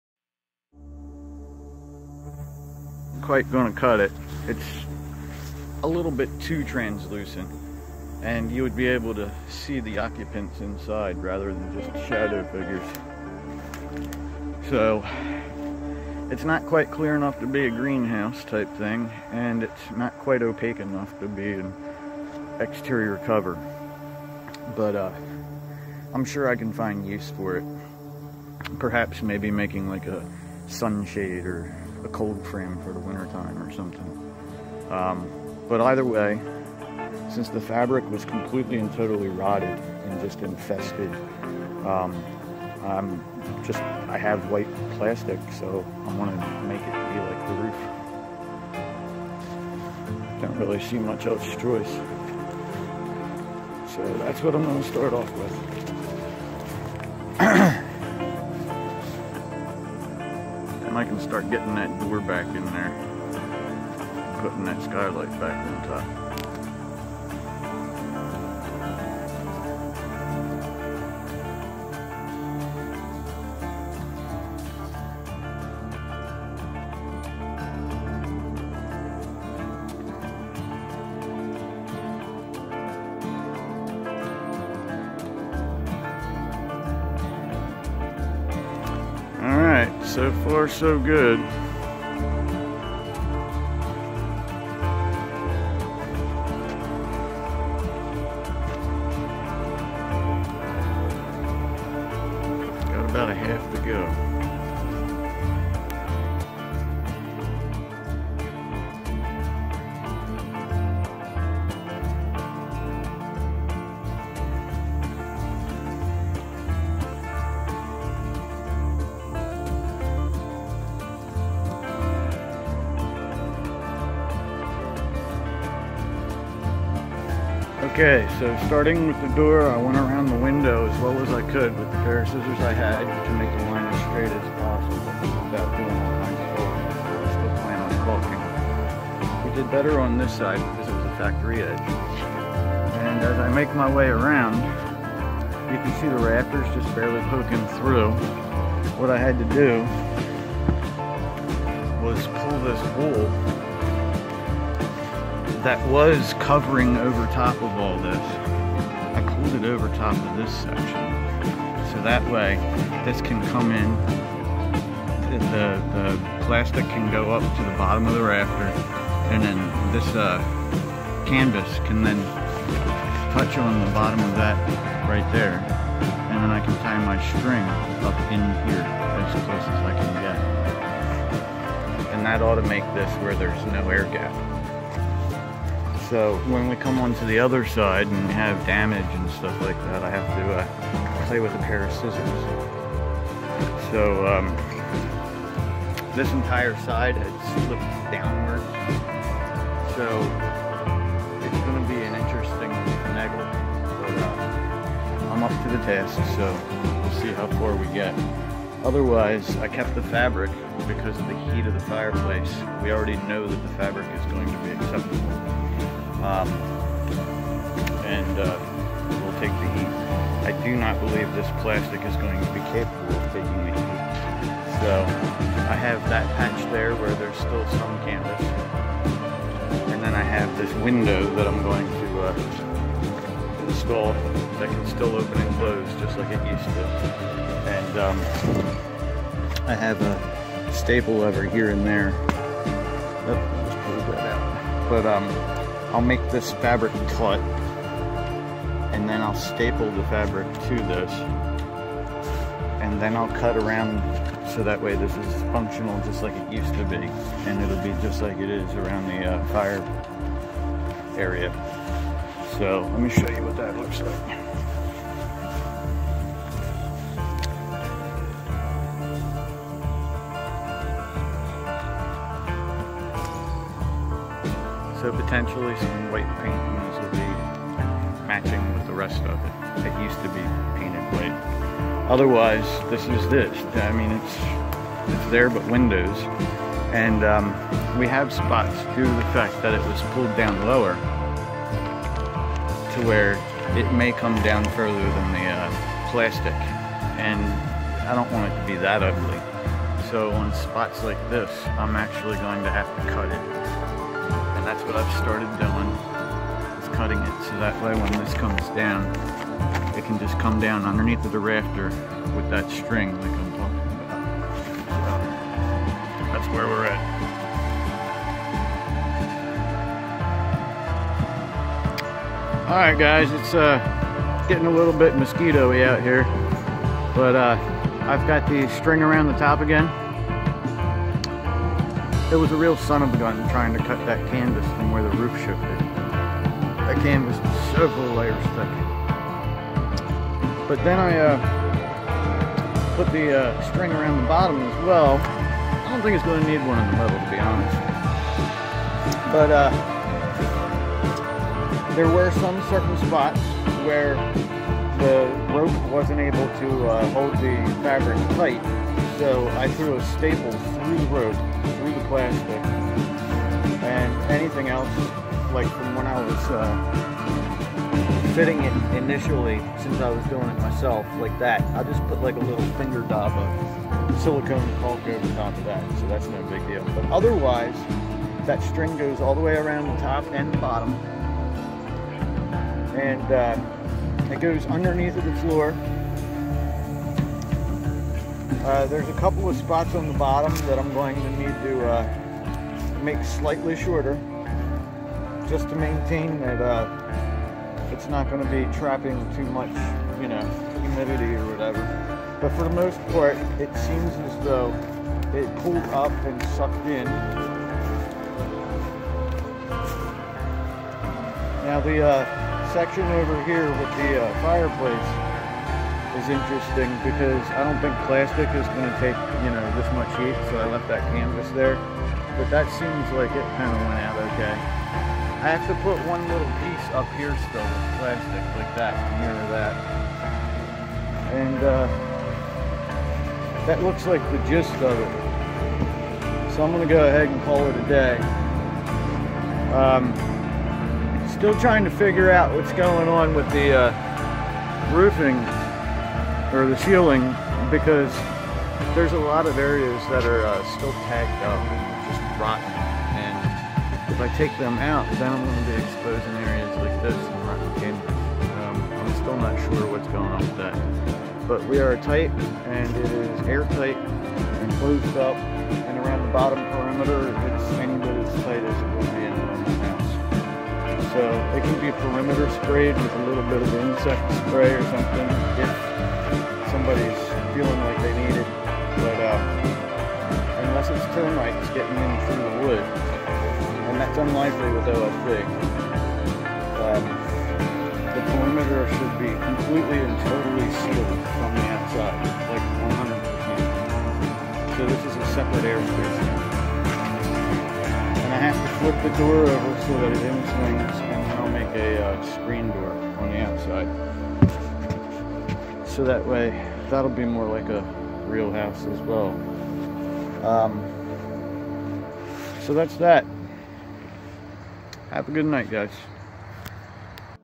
New Yurt Walls & Windows (Day4)
𝕄𝕐 𝕋𝕀N𝕐 H𝕆𝕌𝕊𝔼, 𝕍𝔸N 𝕃𝕀𝔽𝔼, R𝕍, N𝔸𝕋𝕌R𝔼, & 𝕋R𝔸𝕀𝕃𝕊 𝕃𝕀𝔽𝔼 𝔹𝕃𝕆𝔾

Stayed up very late due a surprise rain so today was a bit shorter than typical.

I was however still able to enclose the structure with white tarp plastic sheeting to match the ro...

for more content including videos :)